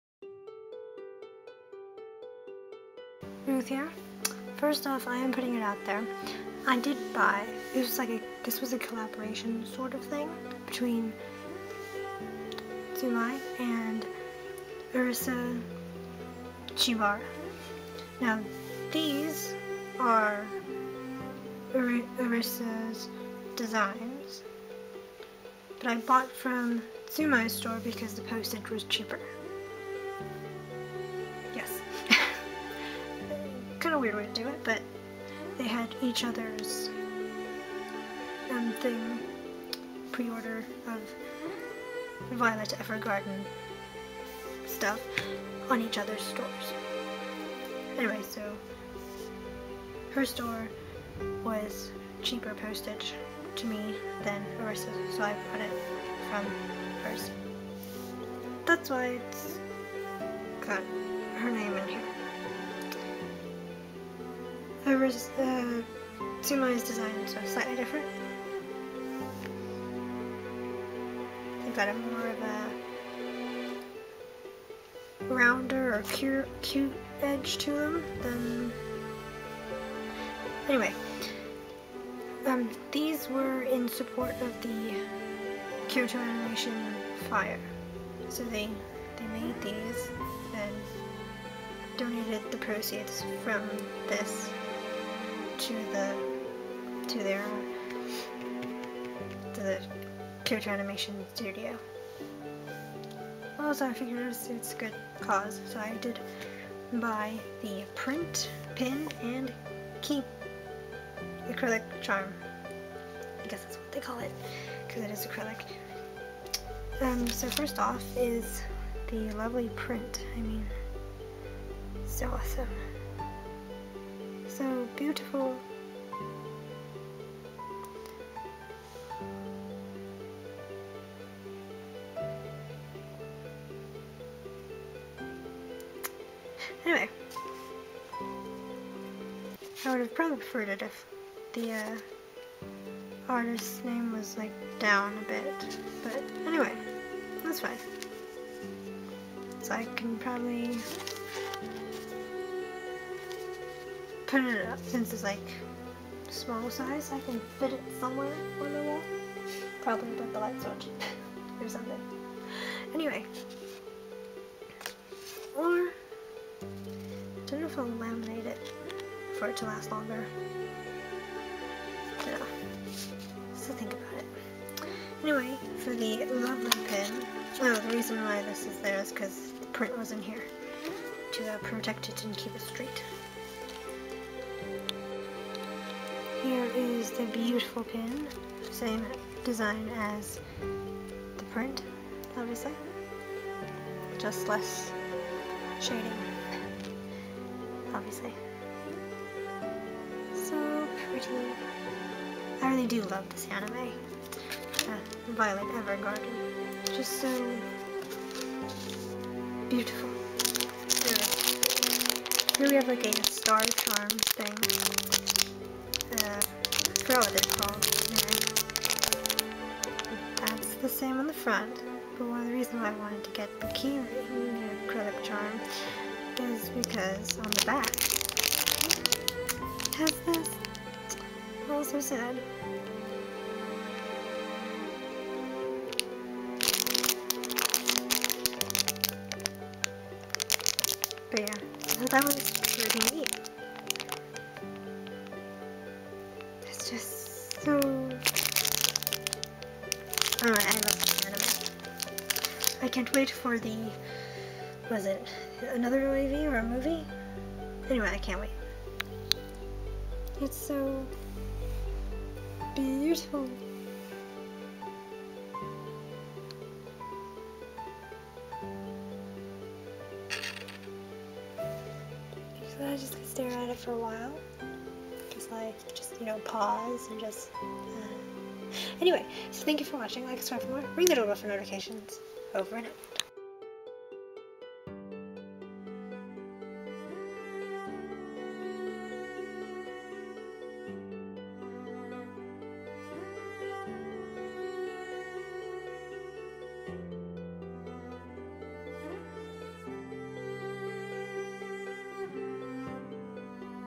Violet Evergarden Pin, Print & Acrylic Charm ~ arisa_chibara
Opening of: Violet Evergarden Pin, Print & Acrylic Charm ~ arisa_chibara

(Both Collab with eachother and sold on each other's store but as explained in the vid I bought from __xuimai's store cause the shipping was cheaper! ^.^ )